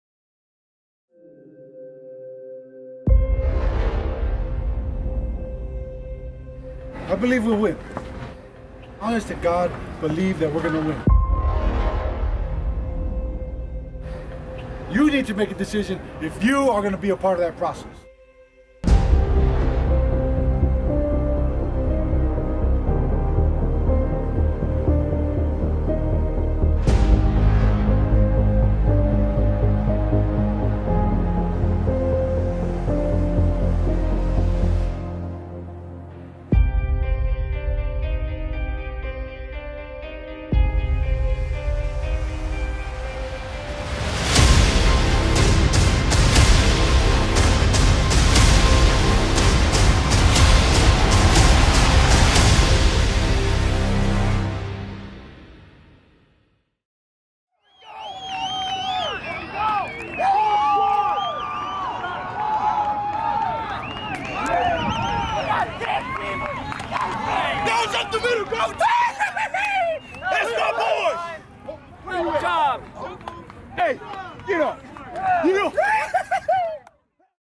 Knights Edge Season 2 Episode 7 Teaser | Southern Virginia University Football vs Apprentice School
Southern Virginia University Football

Knights Edge Season 2 Episode 7 Teaser

Southern Virginia University football traveled to the coast to take on The Apprentice School. Close game but the Knights came out on top!

SVU Football takes on Salisbury at home tomorrow at 1:00pm. The game will be broadcast live.

My Gear:
Camera Kit